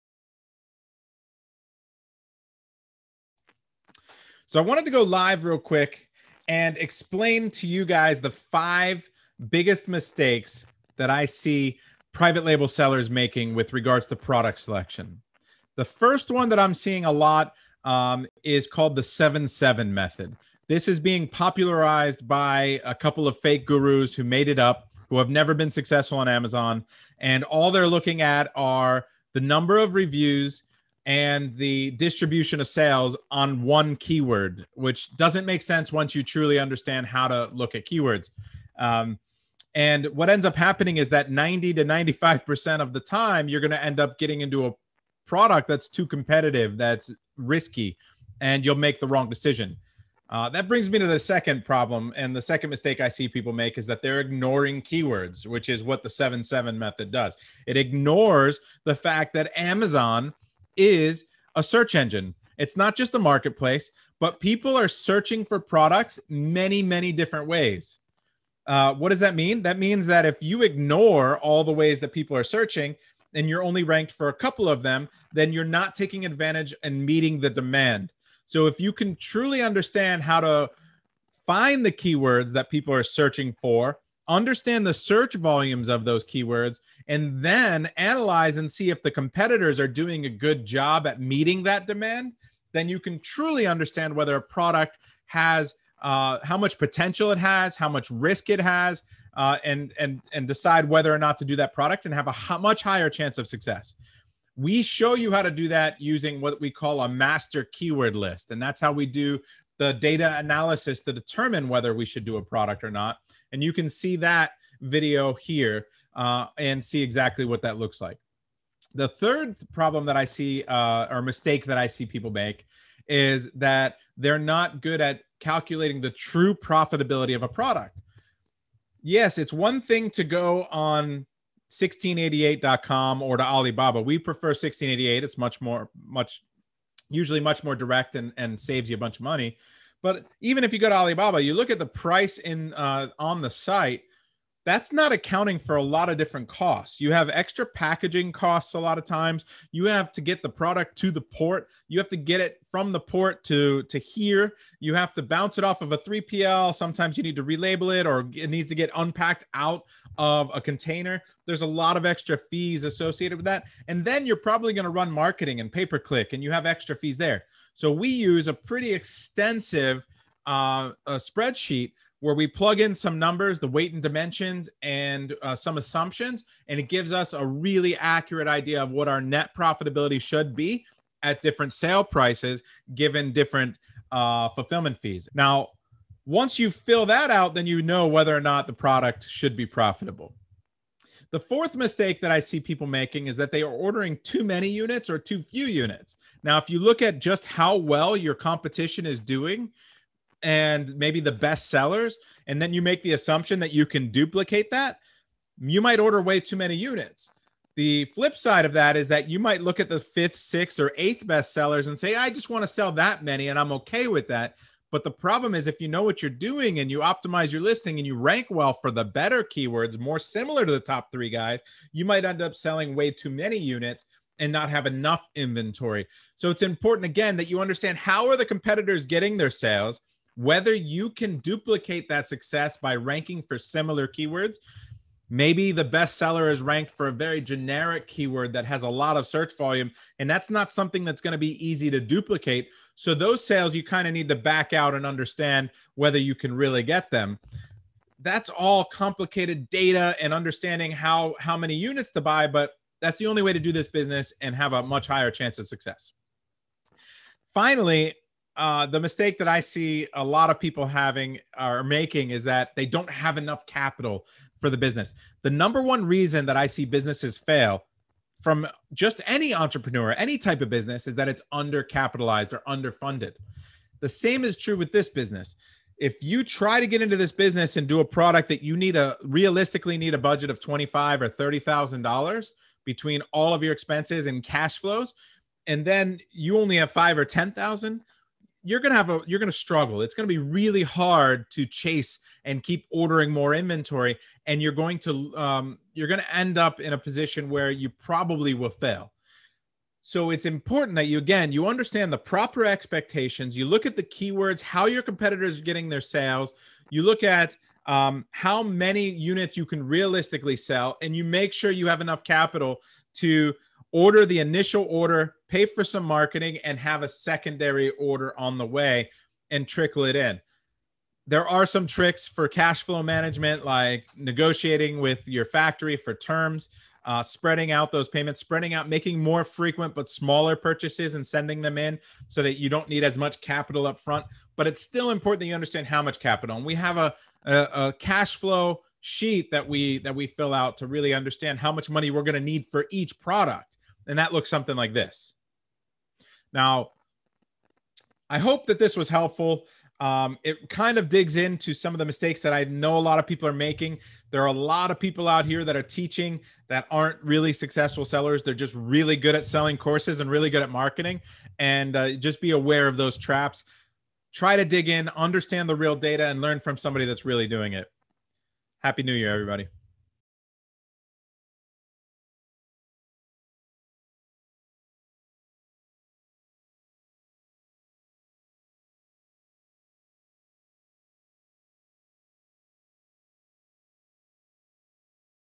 Amazon FBA Product Research 2021 - Top 5 DEADLY Mistakes That Will Destroy Your Amazon FBA Business
The 5 biggest mistakes that I see private label sellers making with regards to product selection are:

1) The 7/7 Method

This has been popularized by a couple of fake gurus who made it up and never been successful with Amazon FBA, as I explain in the video.

2) Ignoring Keywords

When it comes to Amazon FBA product research, we always need to remember that Amazon is a search engine and people are searching for products in many different ways, so with many different keywords.

3) Not Knowing the True Profitability of a Product

We need to take into consideration all the costs properly, otherwise we end up calculating our profitability wrongly.

4) Quantity to Order

Oftentimes people order too many units or too few units, so make sure you watch the video to learn how to fix this.

5) Under Funded

If you try to get into this business and you don’t have enough money to launch a product after you’ve done your Amazon FBA product research, you’re gonna struggle a lot.

So, if you are really serious about becoming a pro at Amazon FBA, go

And get also access to my 5 Hour FBA Masterclass on Product Selection and Optimization